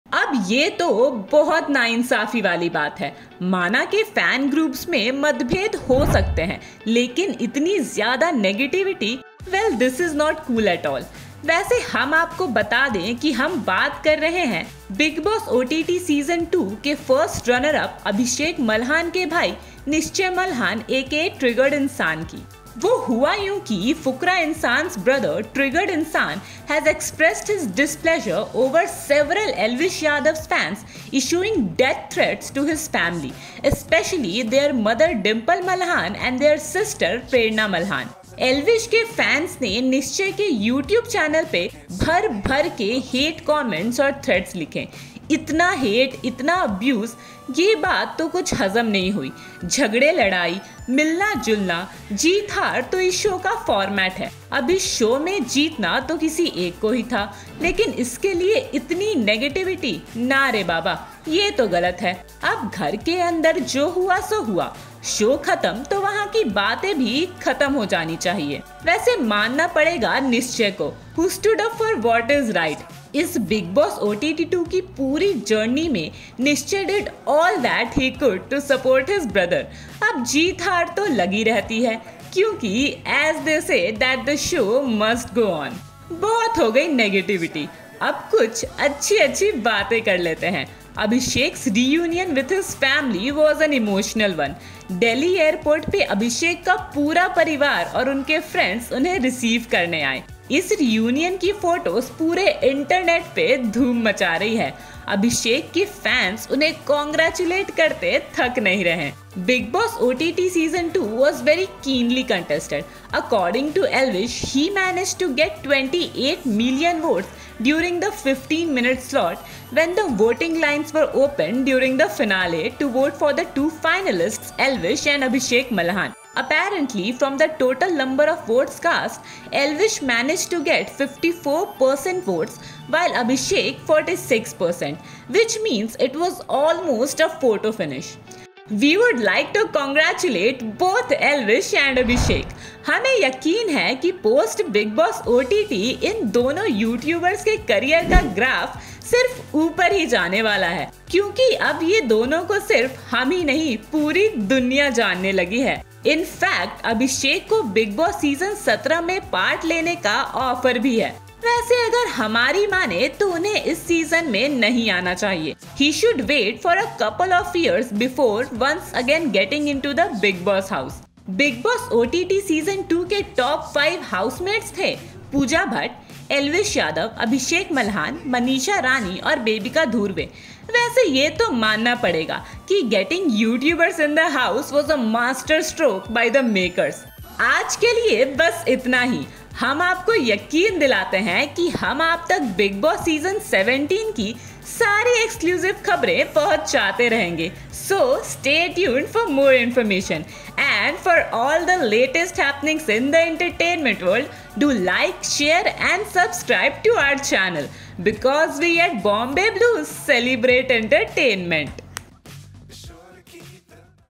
Nischay Malhan Stands Up For His Family As They Receive Death Threats From Team Elvish Yadav
Fukra Insaan's brother Triggered Insaan Aka Nischay Malhan has expressed his displeasure over several Elvish Yadav's fans issuing death threats and hurling abuses to his mother Dimple Malhan and sister Prerna Malhan. Elvish Yadav's fans have been flooding the comments section of Nischay Malhan's YouTube posts, using abusive words for him and his family.

Things got ugly when a few fans issued death, threats to Malhan's family members. They crossed all the lines and used derogatory language. Nishchay tried ignoring these comments, but then when things went out of hands, Nischay stood up for his family and gave it back to team Elvish Yadav, which we feel was required. Now this is really not done. Why drag families into all this. Also, the show is over and so we also need to move on, celebrate the success and let everyone spread love.

The finale saw Pooja Bhatt, Bebika Dhurve, Manisha Rani, Abhishek Malhan and Elvish Yadav in the Top 5 fighting for the trophy. Pooja got evicted in the fifth spot, followed by Bebika and then Manisha with Abhishek and Elvish in the Top 2. Elvish Elvish won the trophy and also Rs 25 lakh cash prize.